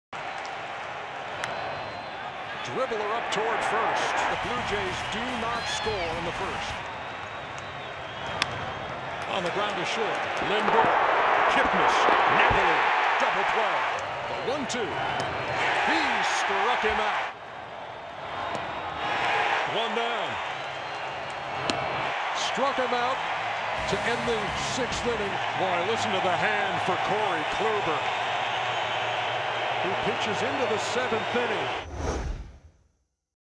ALCS Gm1: Kluber holds Blue Jays scoreless over 6 1/3
Corey Kluber turns in an outstanding start in Game 1 of the ALCS, striking out six while scattering six hits in 6 1/3 scoreless innings